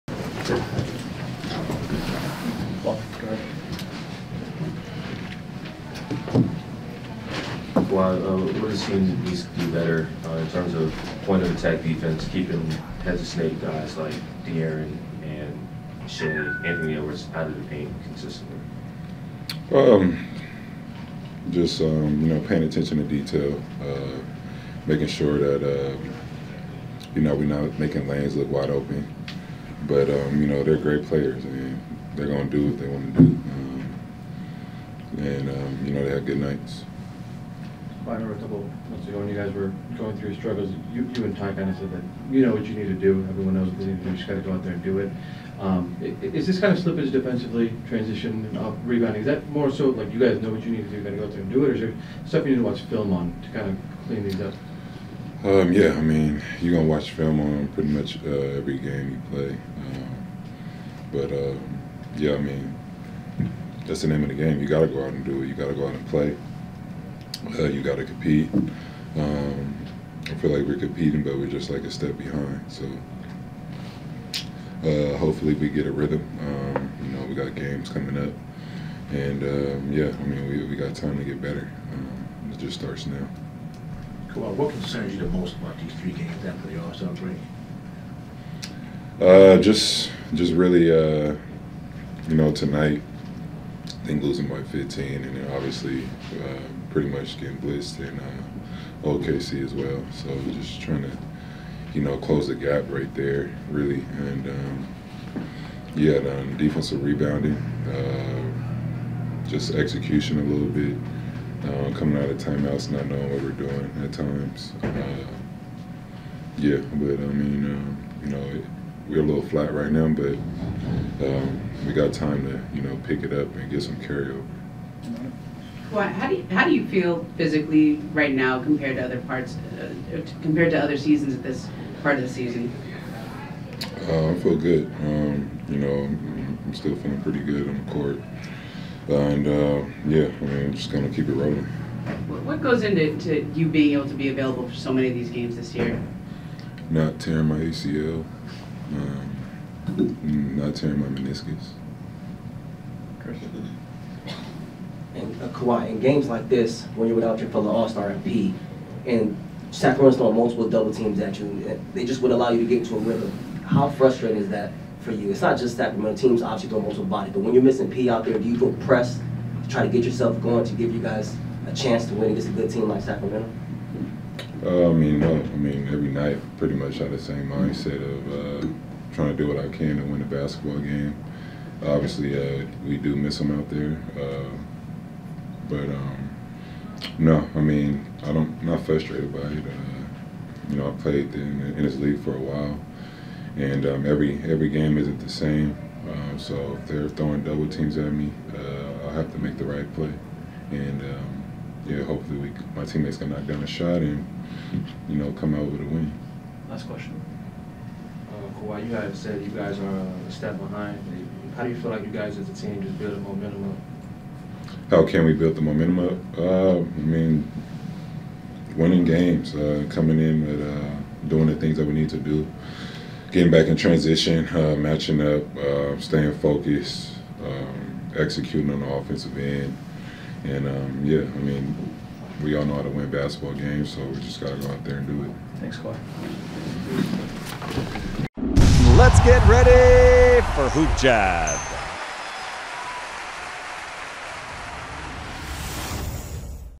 Kawhi Leonard Reacts To The Clippers 123-107 Loss To The Kings. HoopJab NBA
Los Angeles Clippers superstar forward Kawhi Leonard speaks to the media following the Clippers 123-107 loss to the Sacramento Kings.